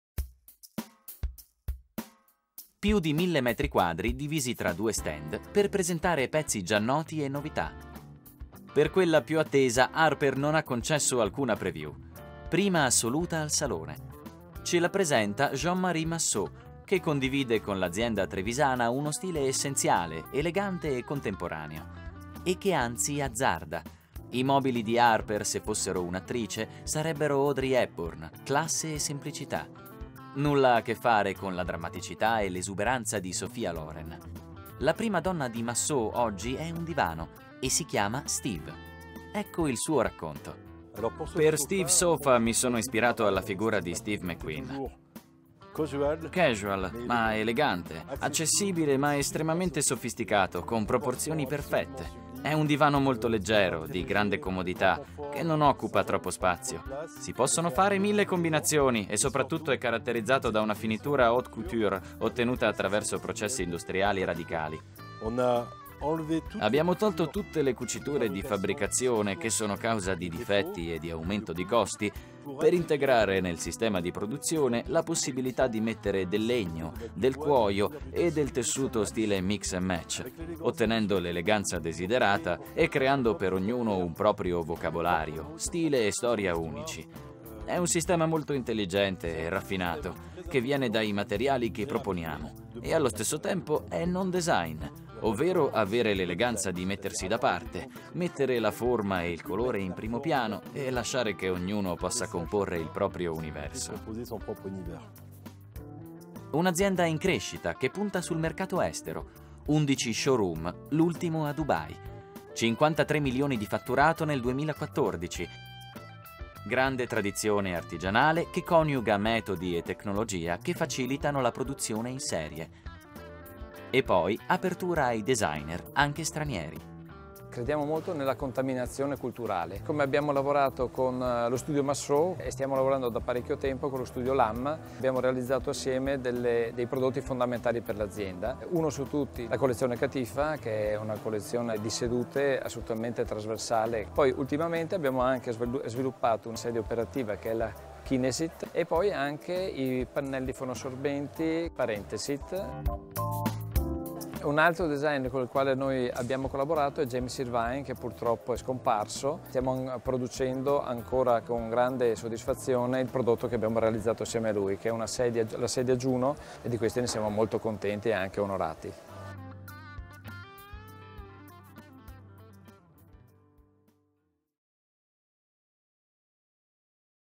Arper, Salone Internazionale del Mobile, 14-19 April 2015
Interview with Claudio Feltrin, Arper CEO, and Jean-Marie Massaud, designer of the new Steeve modular sofa launched at iSaloni 2015